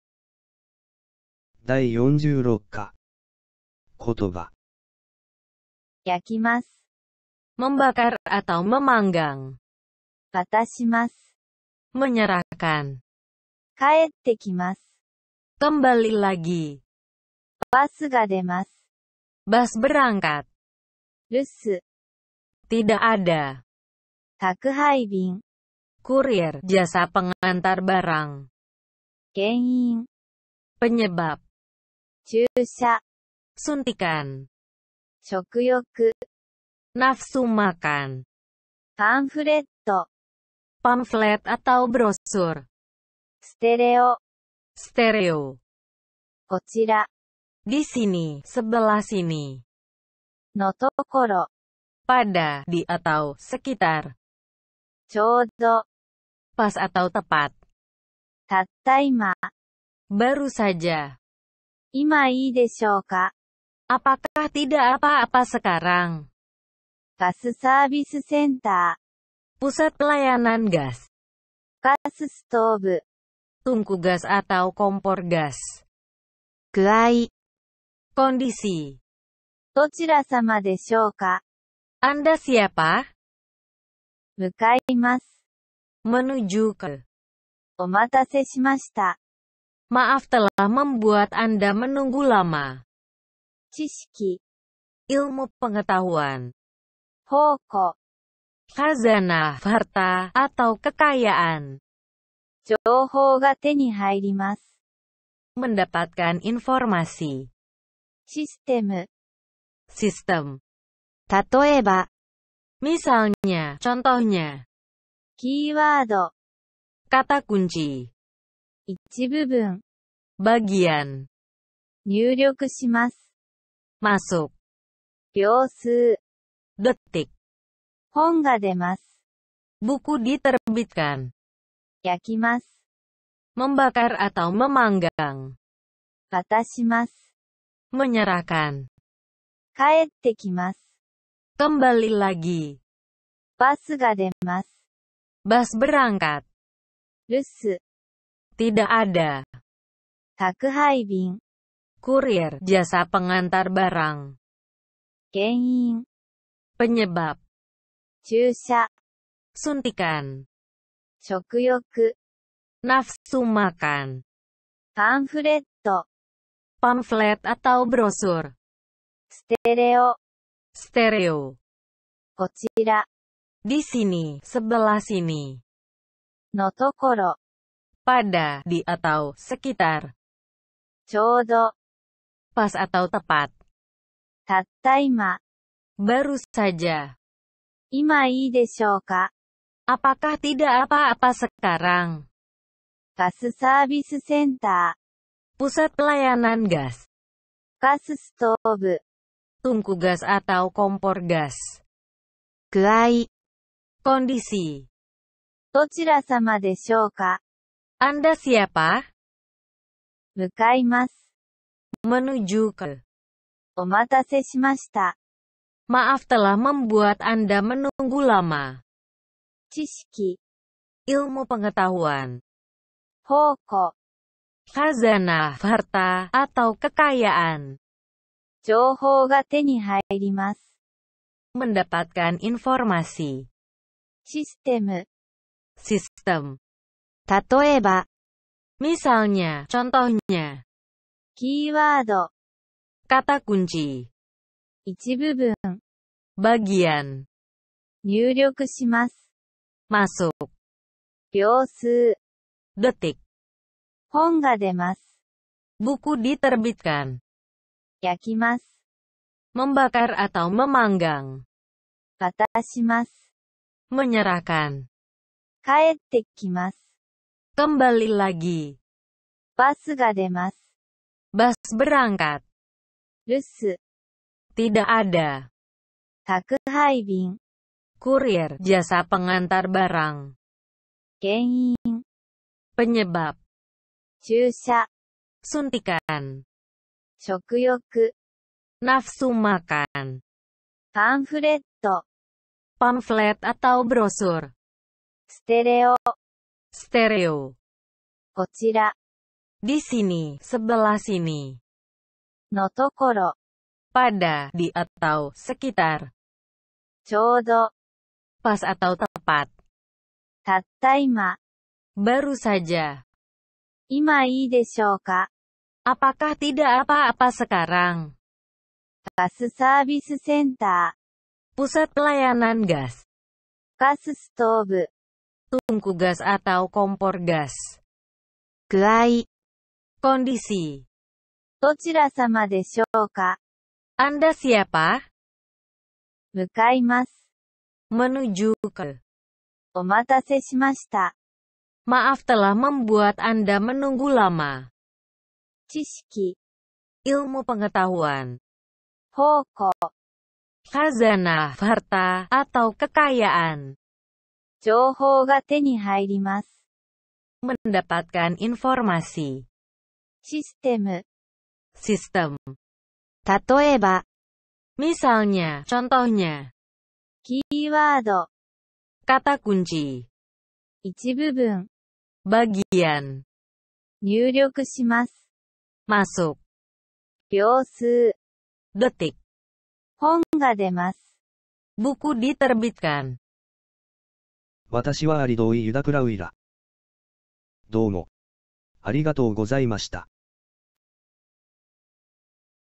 Audio Kotoba Minna No Nihongo Bab 46 versi Bahasa Jepang - Indonesia
Hallo "KONCOMODACHI"
Seri kali ini aku mau share video "Kotoba Minna No Nihongo Bab 46 versi Bahasa Jepang - Indonesia" Sesuai request dari salah satu temen subscriber yang minta supaya audio english dihilangkan biar lebih fokus hafalin kotobanya.  aku buatkan versi audio yang khusus bahasa Indonesia - Jepang aja, semoga bisa membantu temen - temen hafalan kotoba buku Minna no Nihongo bisa di dengarkan sambil menjalankan aktifitas. Selamat menonton & menghafal Semoga videonya bermanfaat, Seperti biasa Mohon koreksi dari temen - temen jika ada kesalahan penulisan, terjemahan, maupun pengucapan.
じゃ, また ね.

keyword ;